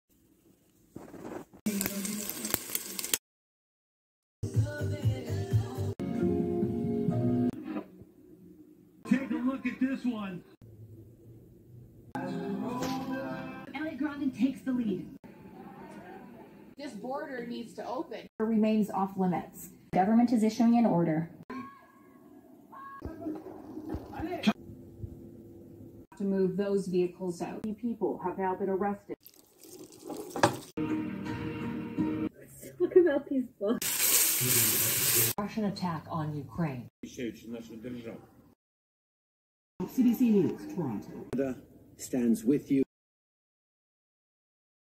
February 1SE 2022 ~ 1 Second Every Day from February 2022
It would be an understatement to say this month was like no other.  I usually share what I'm watching, creating, playing and the sky. This month was a lot of world events, and sadly many of them have been very concerning especially in Ukraine.  My heart goes out to all who had to leave, as well as those there.   If you've watched or read anything about what's going on, you will see the strength, determination and heart already runs very strong.

Here is some information on donations made to the Canadian Red Cross for the Ukraine Humanitarian Crisis Appeal:
"The Government of Canada will match all eligible donations—not exceeding $100,000—made by individual Canadians to the Ukraine Humanitarian Crisis Appeal between February 24, 2022 and March 18, 2022, up to a maximum of $10 million."